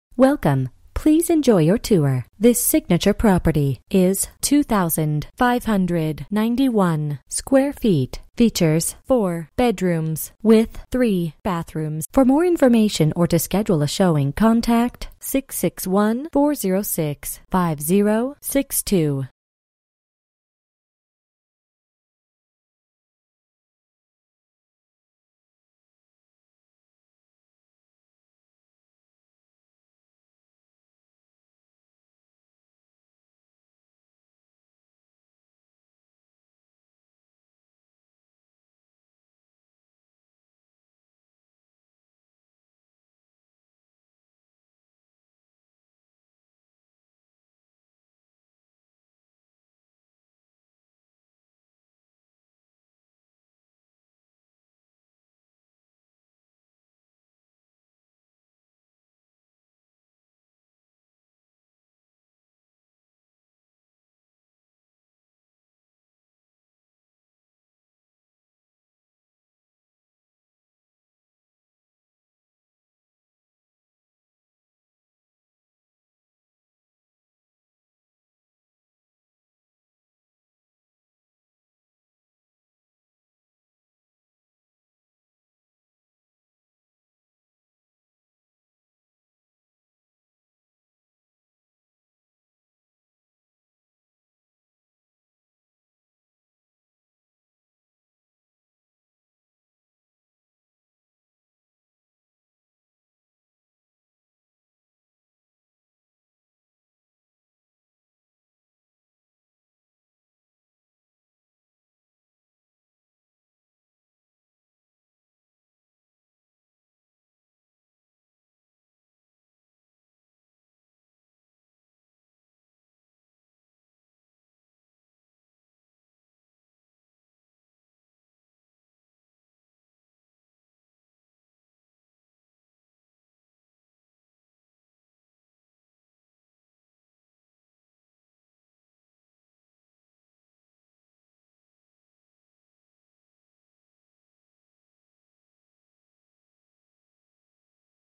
39845 W 86th Street for sale in Leona Valley, CA 93551 - Residential
Beautiful country living!  This 4 bedroom, 3 bathroom home is located on over 2.5 acres of land, with breathtaking views of the rolling hills of Leona Valley. This home features a duel-sided fireplace, a wood-burning stove in the master bedroom, and beautiful wood floors throughout.  The property also has a cell tower which may generate potential future income.

Bedrooms: 4
Bathrooms: 3
Square Feet: 2591
Price: $550,000